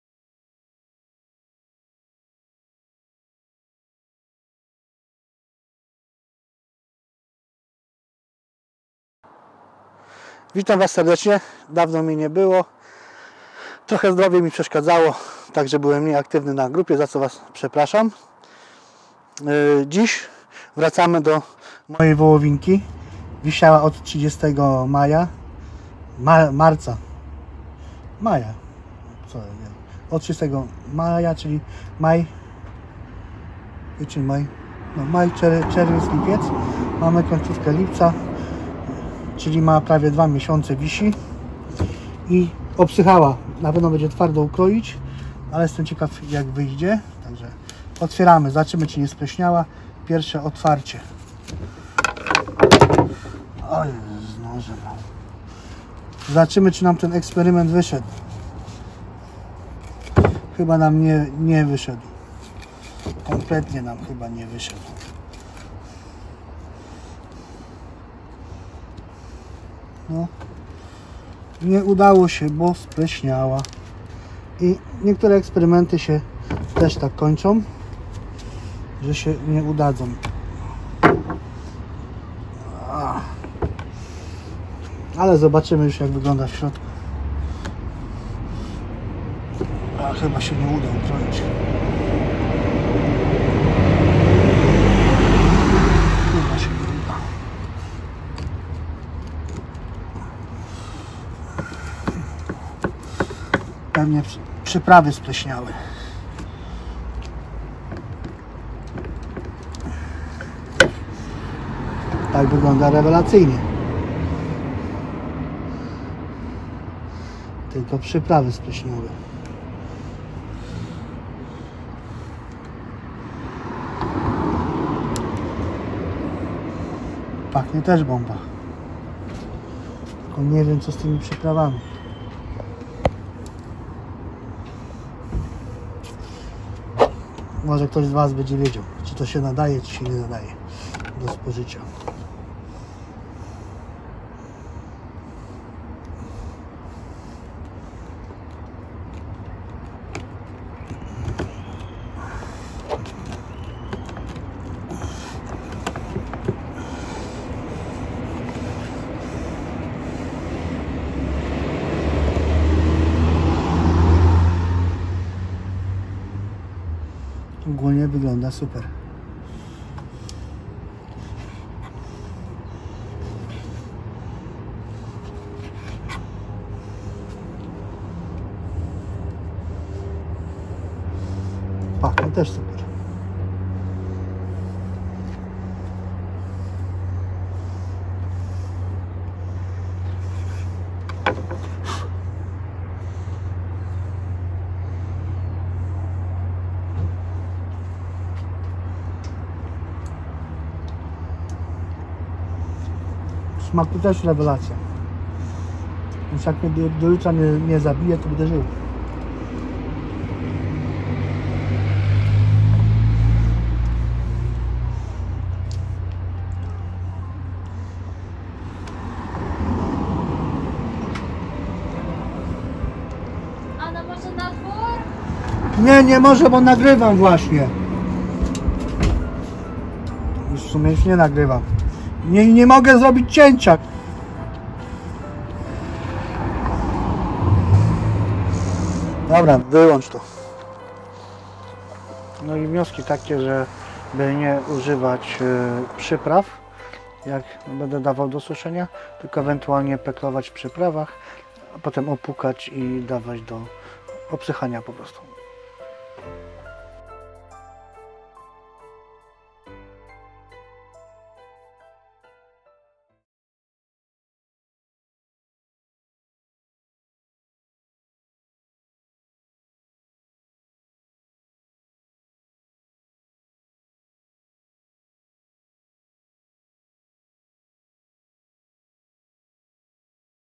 Wołowina dojrzewająca Odcinek 4
Wołowina Silver Eye

czosnek niedźwiedzi
majeranek
pieprz kolorowy
sól kłodawska nie jodowana ( można użyć innej soli również soli do peklowania)